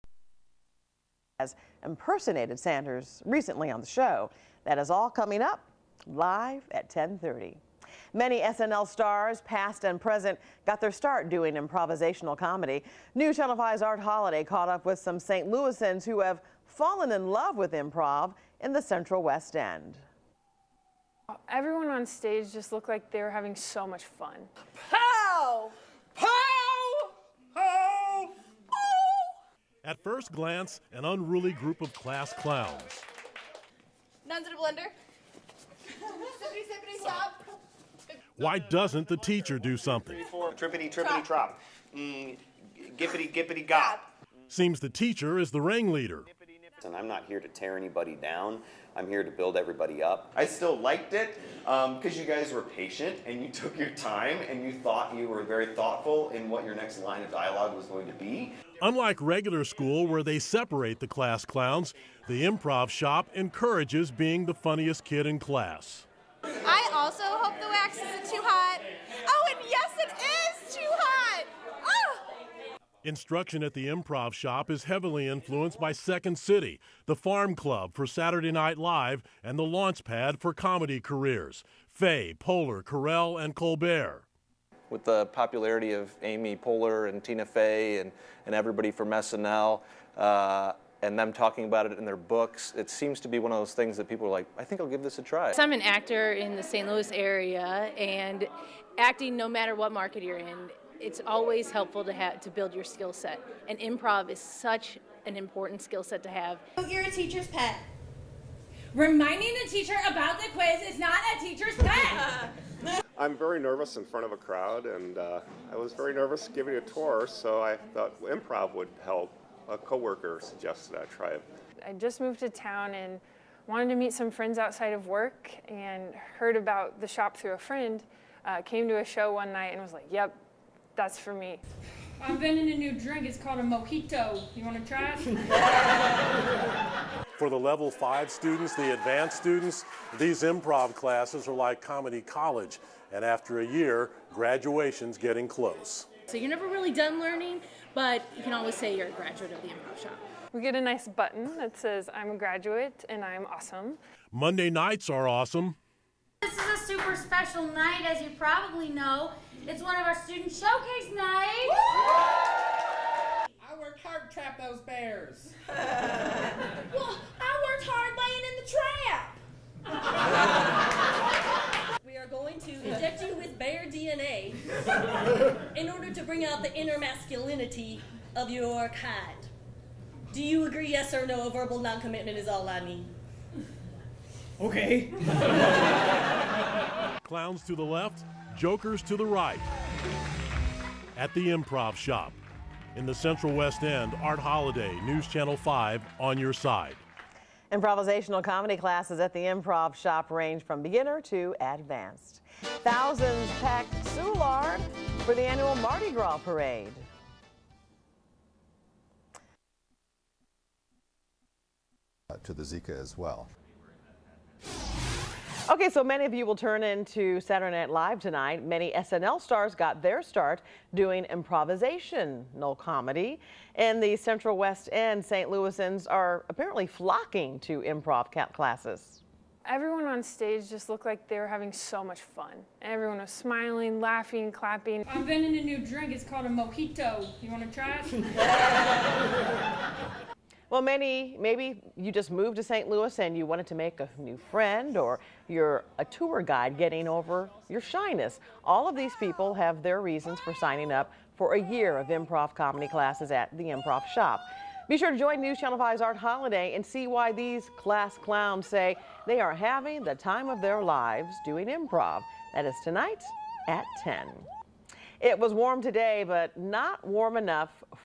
Cara Artman--"The Improv Shop" news story on KSDK
Thank you GOD for the opportunity to appear in this news story to brag about "The Improv Shop"! The news story and then a promo are included on the video.